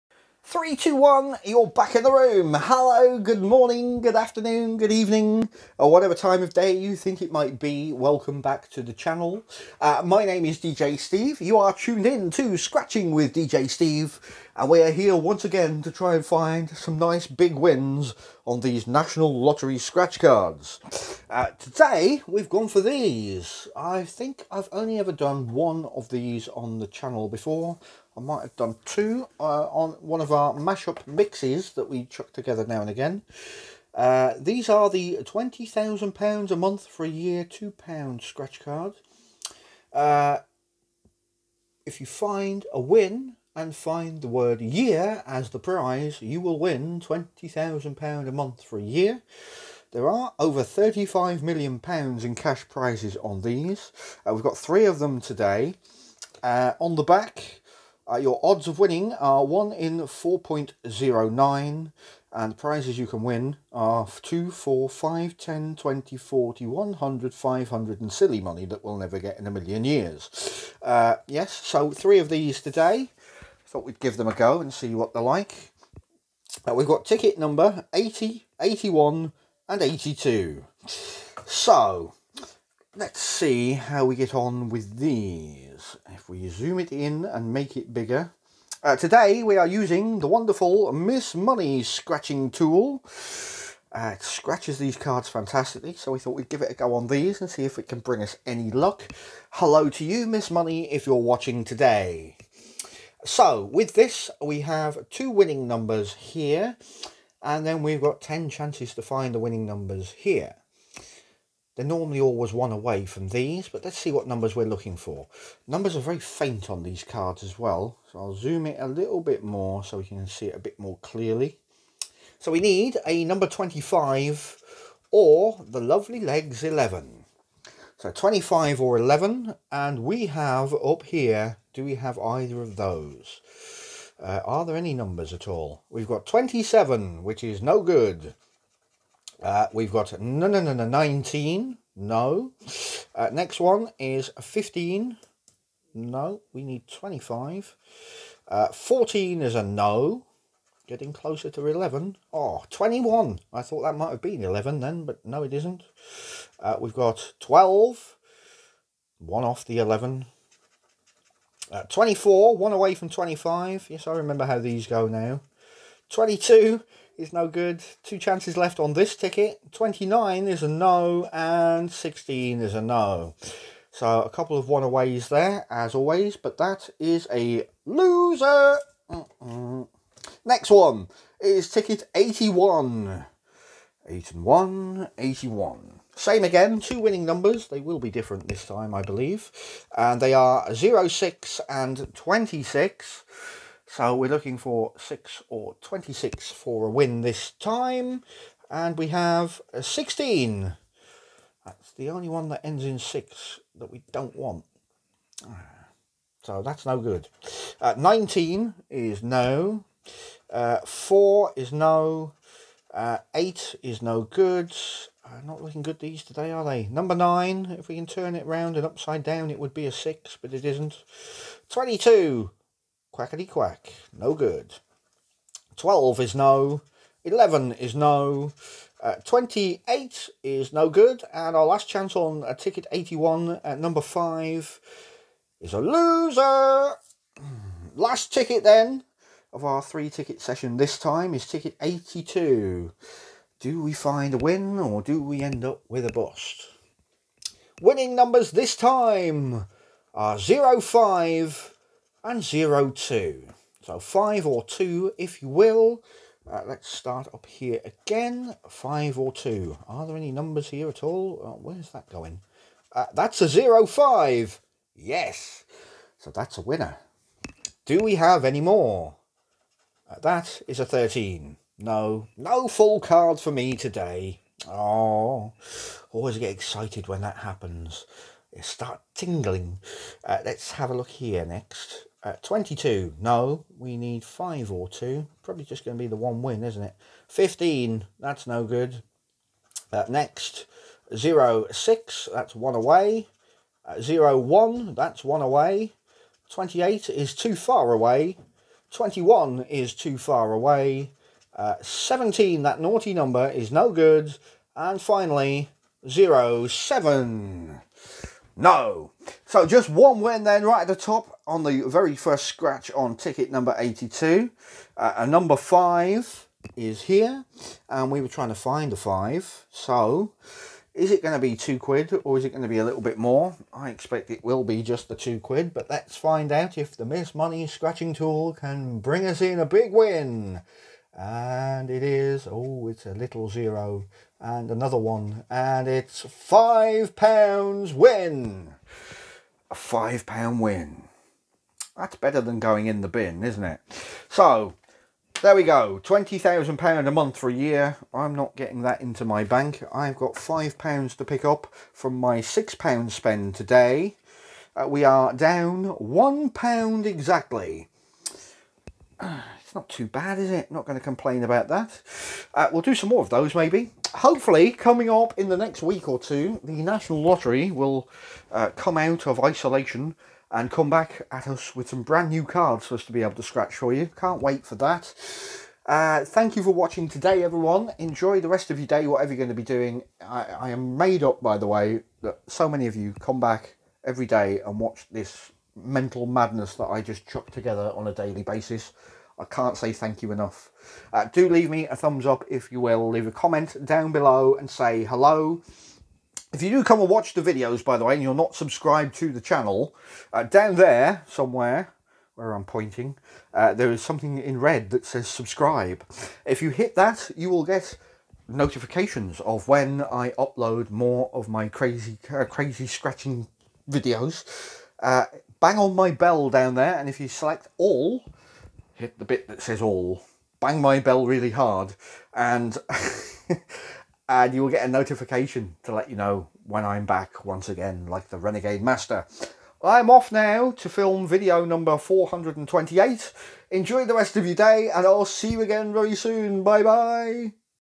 Friday Sesh! Win Or Bust? 🤞🤞🤞
Happy Friday! Today we try 3 of the 20,000 a month for a year tickets! Can we find a win today or do we have a bust? 🤞🤞🤞